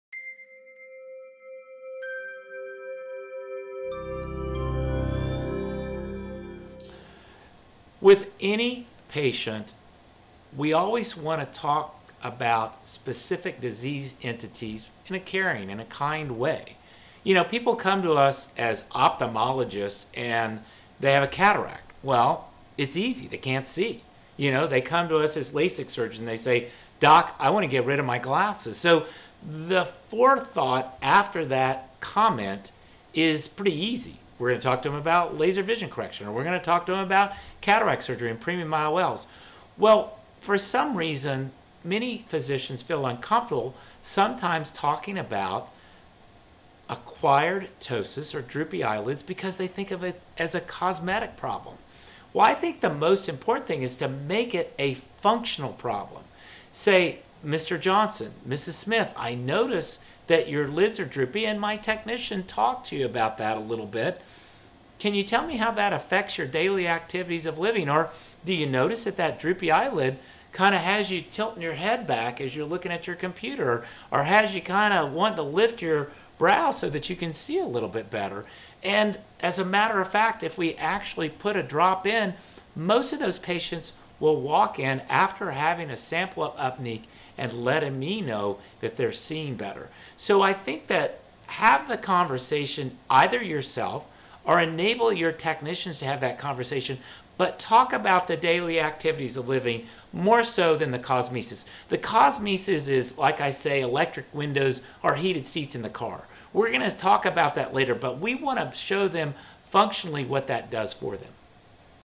How do you talk to your patients about acquired blepharoptosis?
Karl Stonecipher, MD
Clinical Professor of Ophthalmology,
University of North Carolina, Chapel Hill
Clinical Adjunct Professor of Ophthalmology,
Tulane University, New Orleans
Director of Refractive Surgery, Laser Defined Vision,
Greensboro, North Carolina